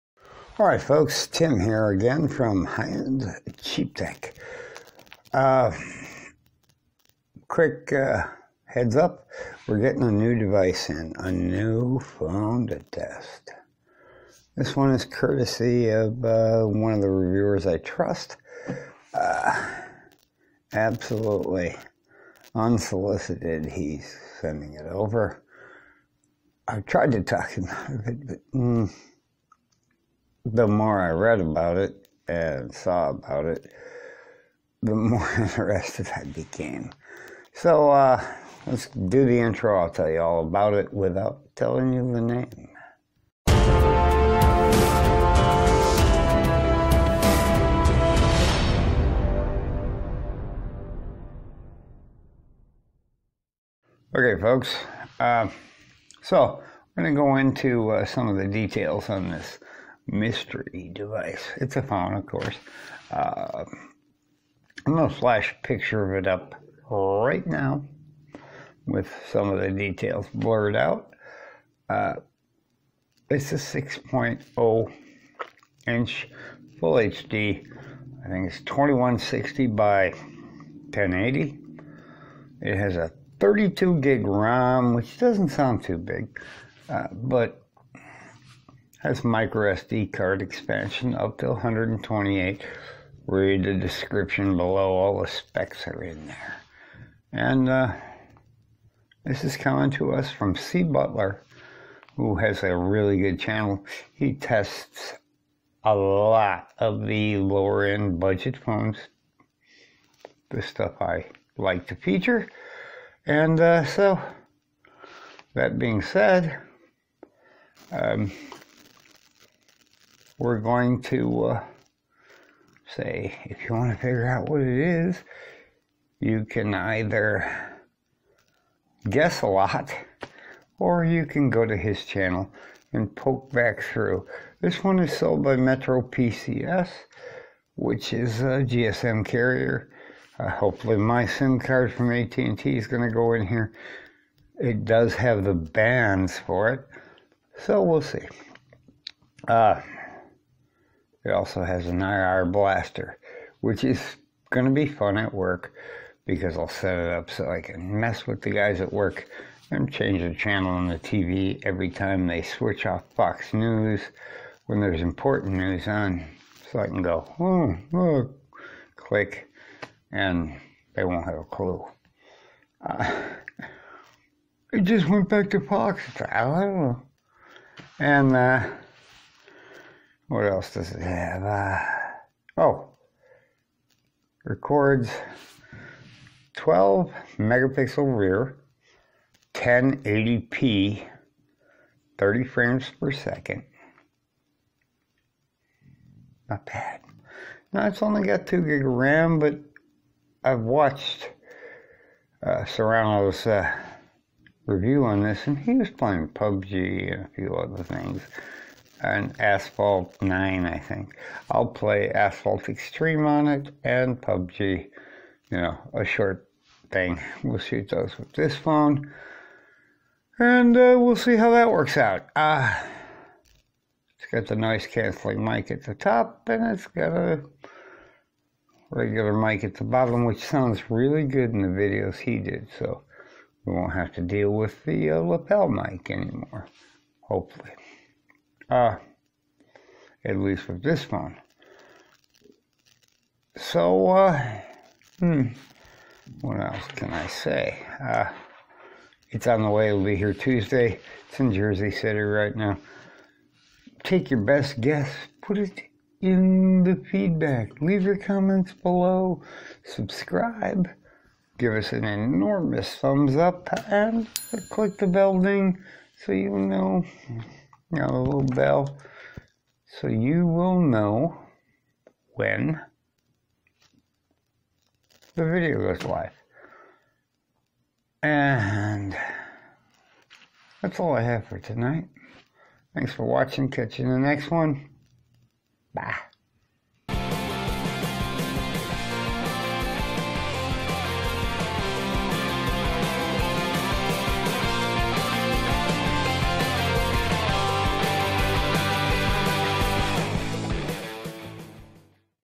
A Fresh Test Phone Arriving Tuesday!
We have a new device on the way! I am not going to spoil the surprise, but I will give you all the specs below. This one is going to be very interesting and probably surprise a few folks with its performance!

Specs:
Screen size: 6.0 inch, 1080 x 2180 pixels, 18:9 ratio (~402 ppi density)
Camera: Rear-12 mp, front 8.0 mp
Video: 1080p 30 fps with EIS!
Battery: 4000 mAh
ROM: 32 GB, expandable via microSD, (up tp 128 GB)
RAM: 2 GB
SoC: Mediatek MT6763T Helio P23 (16 nm) @2.5 gHz
GPU: Mali-G71 MP2
IR Blaster: Yes

This video was reduced in size from 294 MB to 66.5 MB using Handbrake